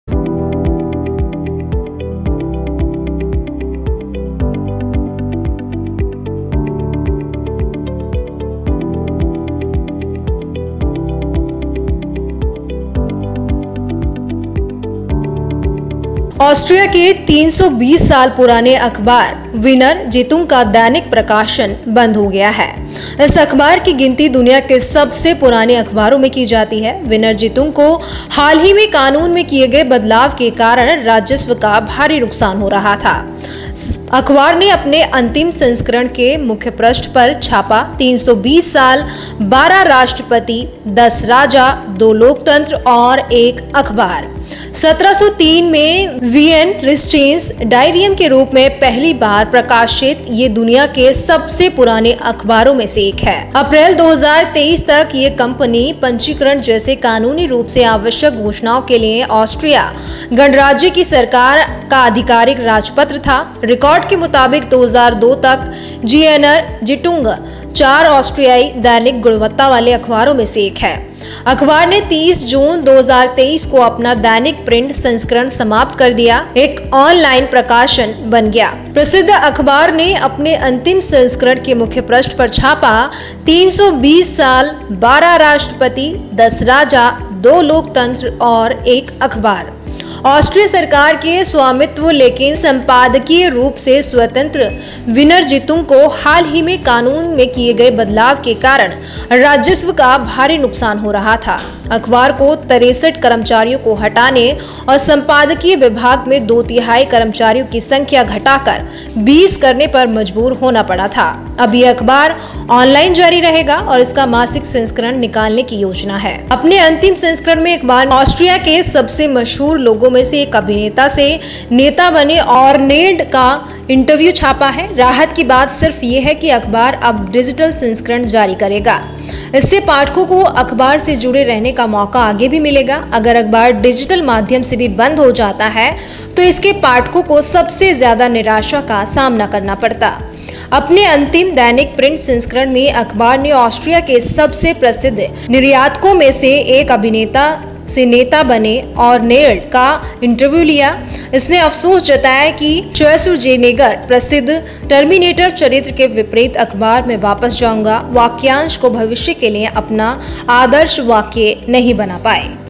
320 साल पुराने अखबार 'वीनर जैतुंग' का दैनिक प्रकाशन बंद | AUSTRIA | NEWS PAPER
austria newspaper WienerZeitung NewsWorldIndia NwiLive LatestNews HindiNews hindinews Austria's 320-year-old daily newspaper Wiener Zeitung has ceased publication. This newspaper is counted among the oldest newspapers in the world. Wiener Zeitung was suffering a huge loss of revenue due to recent changes in the law. The newspaper printed on the front page of its last edition – 320 years, 12 presidents, 10 kings, two democracies and one newspaper.